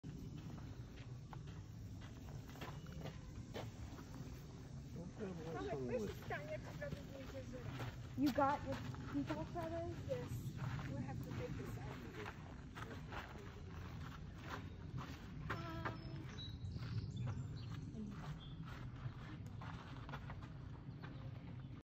Giraffe close-up
I visited the Giraffes at Southwick Zoo in Mendon, Massachusetts. They came right up to me.